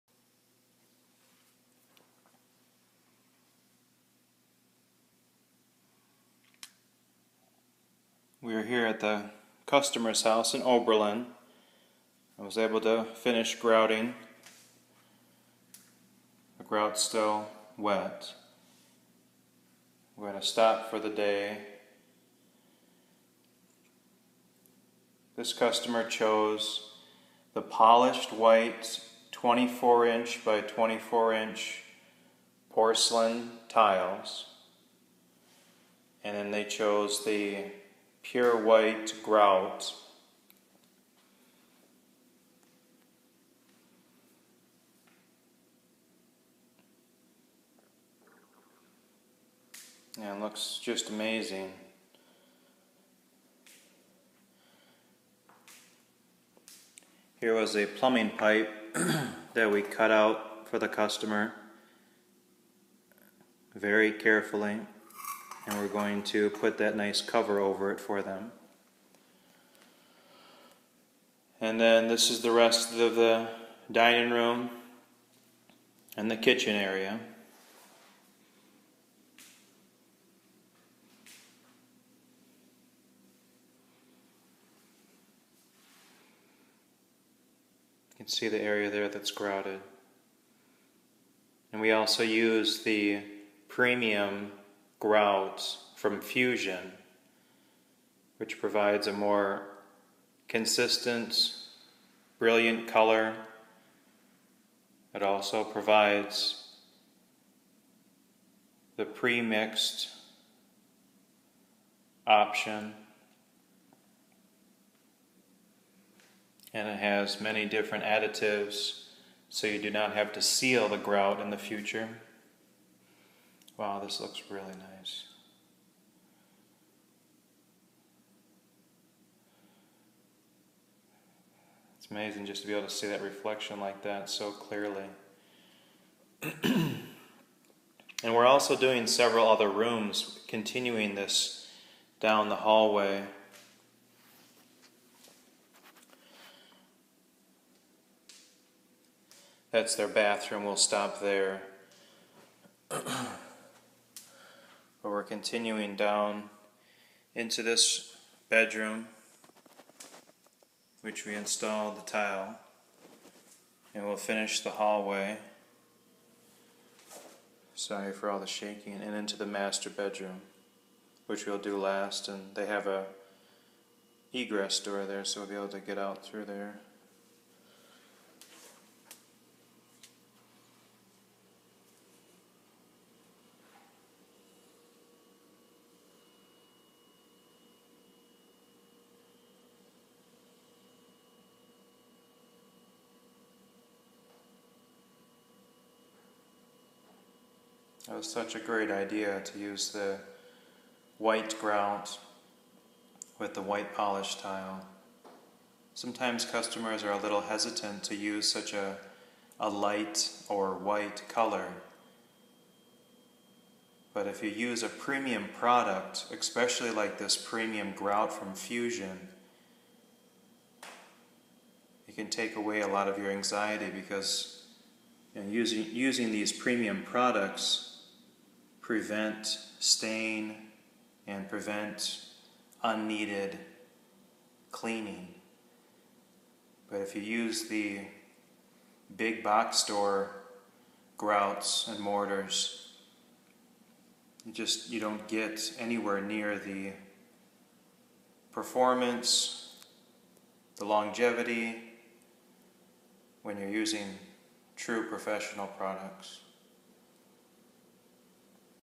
Finished Emser White polished 24"x24"
Finished Emser White polished 24"x24" tile and grouting  throughout entire home.